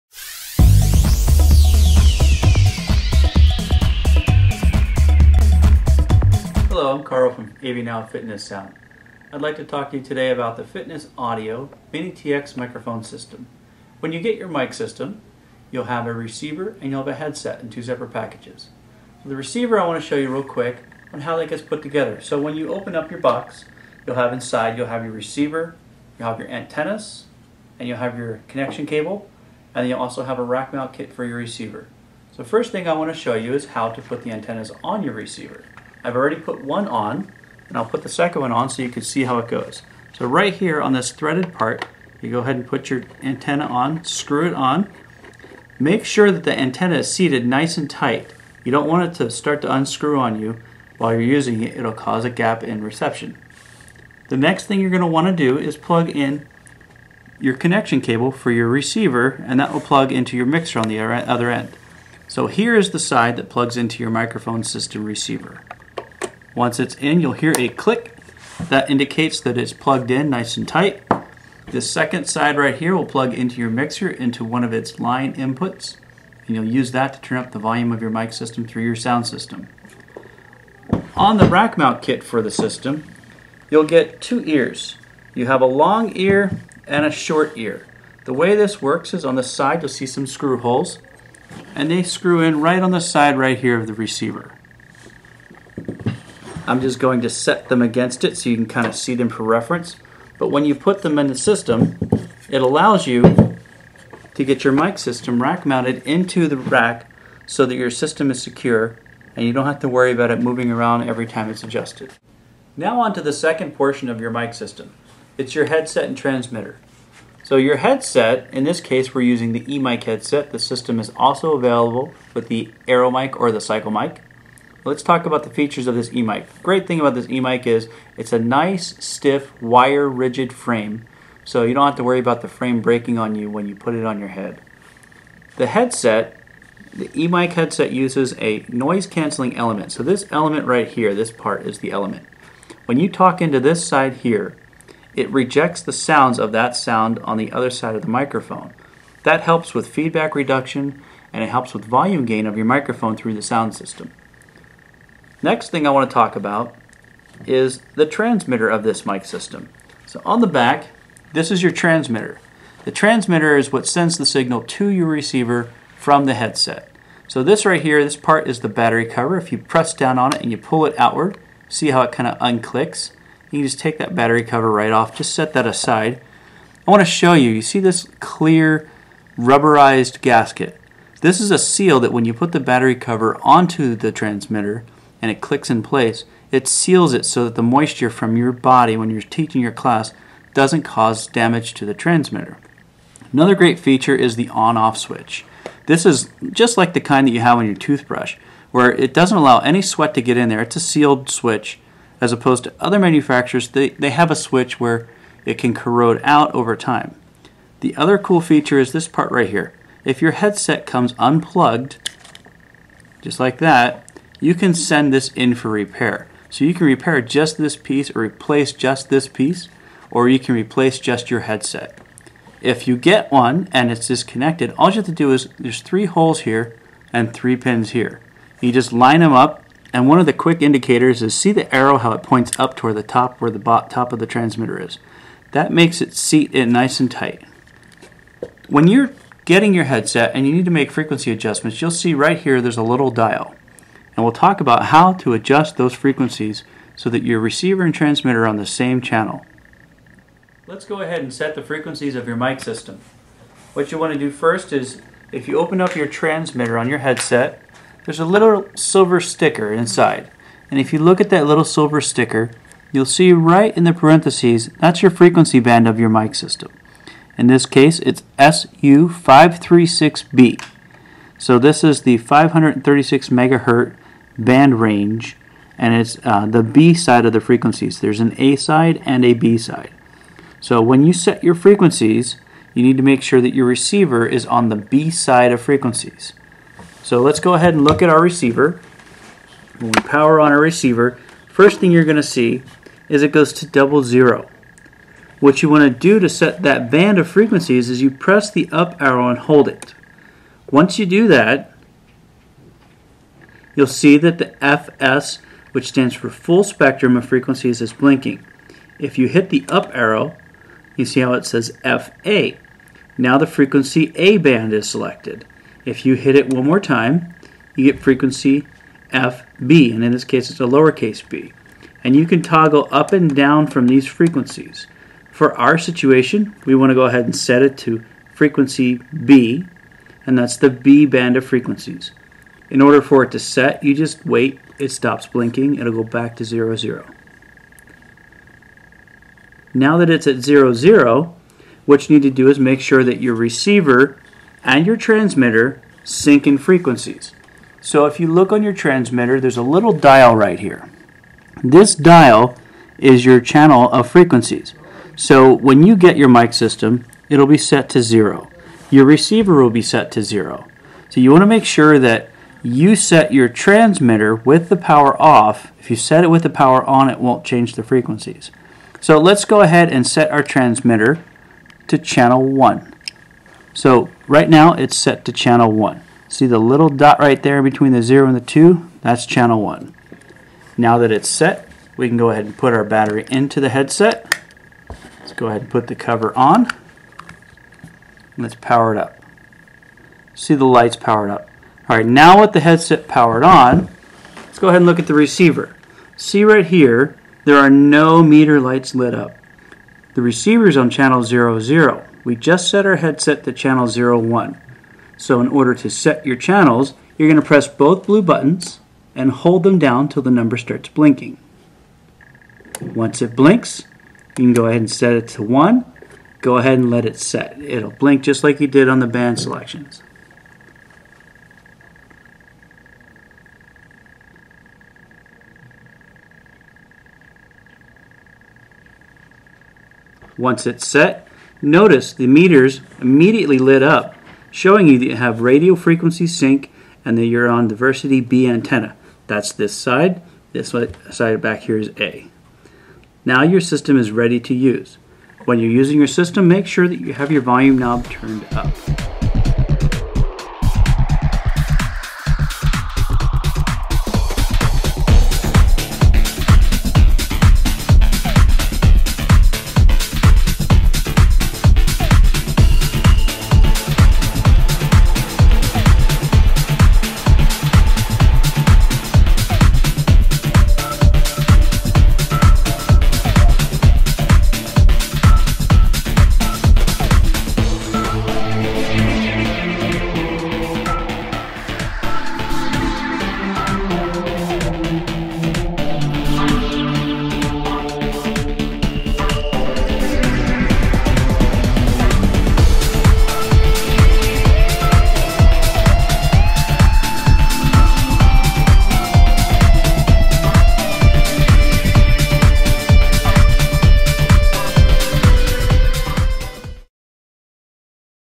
How to set up your Mini-TX wireless microphone system with instructions - AVNow.com
Instructions for Mini-TX wireless microphone system by AV Now Fitness Sound.

NO BELT PACK. FITNESS RATED DURABILITY. Transmitter and headset only. Built for fitness (up to 20 classes per week) with a single SEALED on/off switch, coated circuit board, NO BELT-WORN TRANSMITTER. 8 channel selectable for those cities that have crowded airwaves or for facilities that want to have a single transmitter as a spare.
Uses one AAA battery
For fitness use, up to 20 classes per week
One AAA battery should supply over 5 hours of airtime
Re-chargeable AAA batteries can also be used
Warranty: 1 Year
View Brochure: E-mic Specs
Setup and use info: MT-U8 Transmitter User Manual
Includes the headset and transmitter only, and please double-check your frequency requirements before ordering. We're here to help.

*Your transmitter will need to match the frequency of your receiver.
- If the dial is set to 0 through 8 (the red side) you need A
- If the dial is set to 9-F (the blue side) you need B
- Receiver sold separately